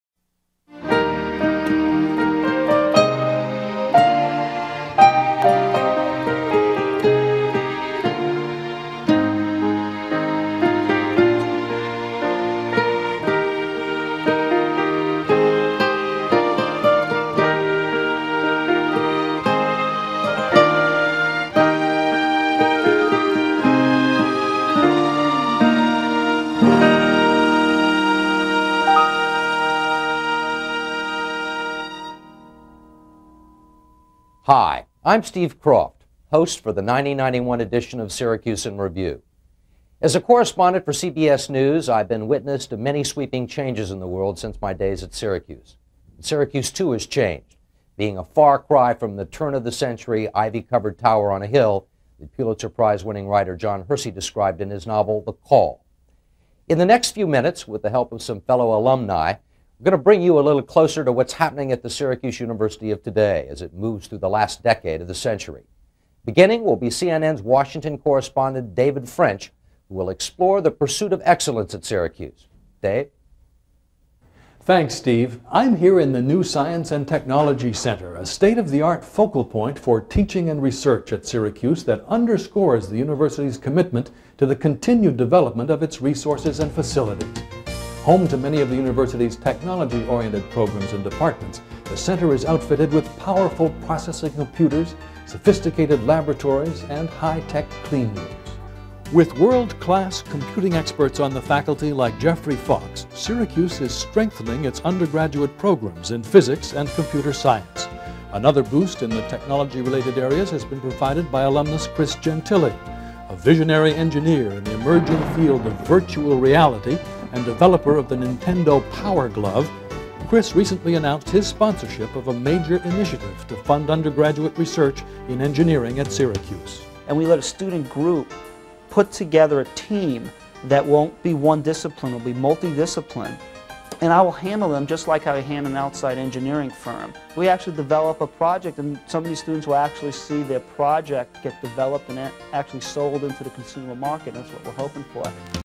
Syracuse In Review IV
This is a clip of the 1991 "Syracuse In Review" yearly video for 1991, featuring Christopher Gentile